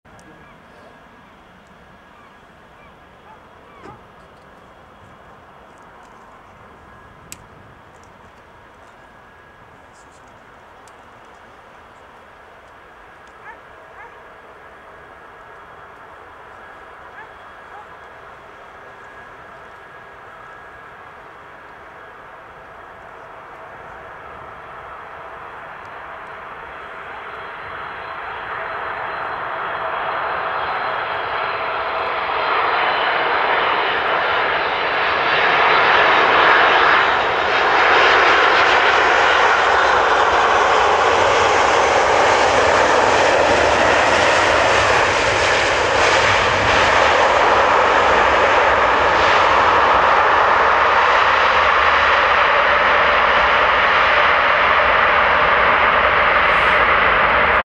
747 LANDING .Cargolux Boeing 747-8F (stretched JUMBO) landing
Cargolux Boeing 747-8F landing at Prestwick, Scotland